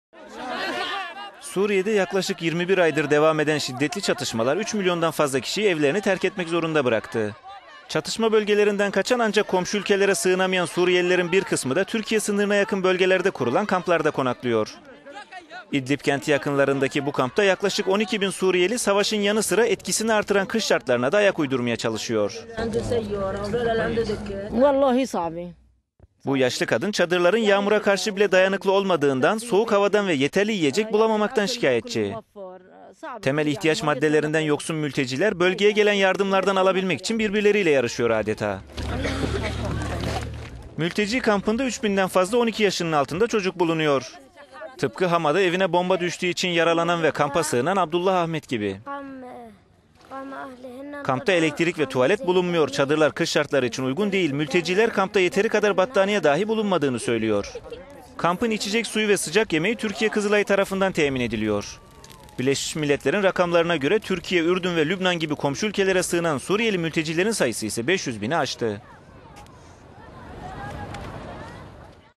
Kış Suriyeli mülteciler için sert geçiyor
Suriye'de yaklaşık 21 aydır devam eden şiddetli çatışamalar 3 milyondan fazla kişiyi evlerini terketmek zorunda bıraktı.

Çatışma bölgelerinden kaçan ancak komşu ülkelere sığınamayan Suriyelilerin bir bir kısmı da Türkiye sınırına yakın bölgelerde kurulan kamplarda konaklıyor.

İdlib kenti yakınlarındaki bu kampta yaklaşık 12 bin Suriyeli, savaşın yanı sıra etkisini artıran kış şartlarına da ayak uydurmaya çalışıyor.

Bu yaşlı kadın, çadırların yağmura karşı bile dayanıklı olmadığından, soğuk havadan ve yeterli yiyecek bulamamaktan şikayetçi.

Temel ihtiyaç maddelerinden yoksun mülteciler bölgeye gelen yardımlardan alabilmek için birbirleriyle yarışıyor adeta.

Mülteci kampında üç binden fazla 12 yaşının altında çocuk bulunuyor. Tıpkı Hama'da evine bomba düştüğü için yaralanan ve ailesiyle kampa sığınan Abdullah Ahmed gibi.

Kampta elektrik ve tuvalet bulunmuyor. Çadırlar kış şartları için uygun değil. Mülteciler kampta yeteri kadar battaniye dahi bulunmadığını söylüyor.

Kampın içecek suyu ve sıcak yemeği Türkiye Kızılayı tarafından temin ediliyor.

Birleşmiş Milletler'in rakamlarına göre Türkiye, Ürdün ve Lübnan gibi komşu ülkelere sığınan Suriyeli mülteci sayısı ise 500 bini aştı.